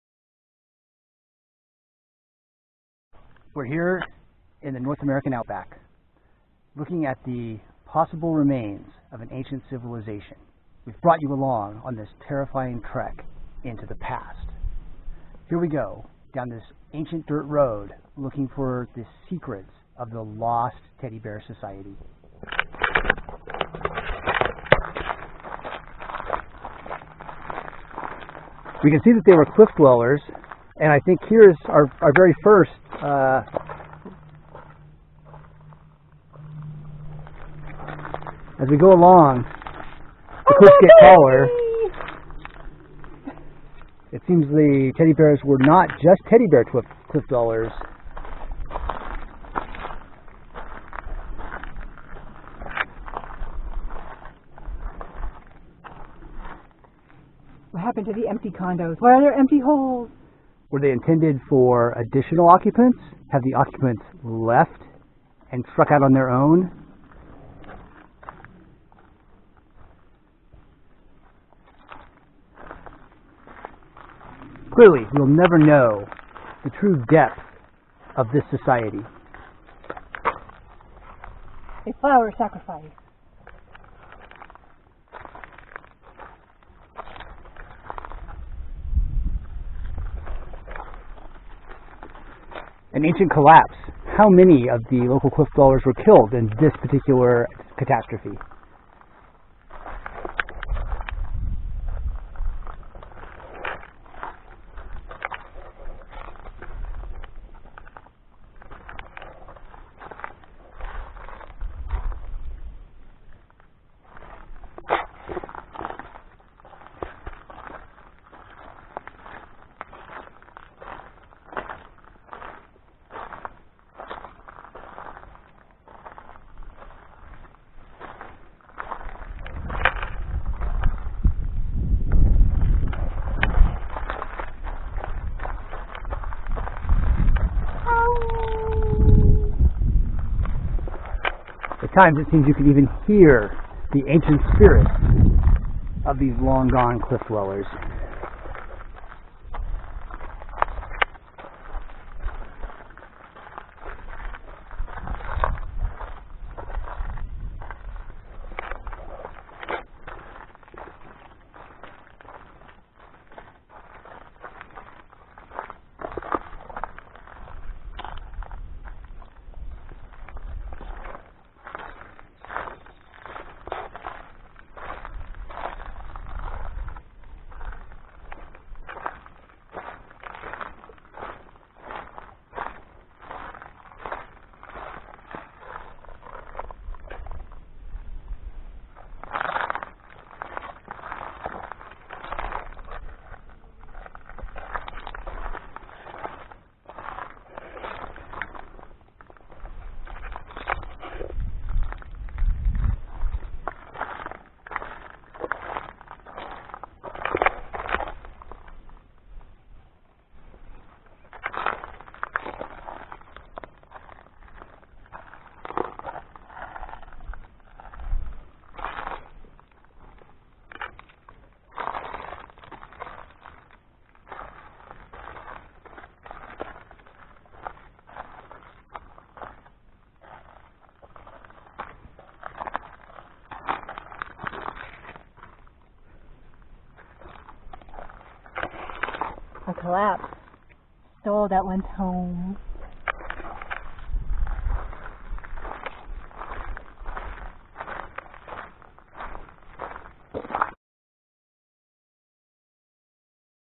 Mysteries of the Mysterious Episode 1
Production values, we have none. Join us in a 4 minute exploration of the mysterious cliff dwellers of Sloan Canyon.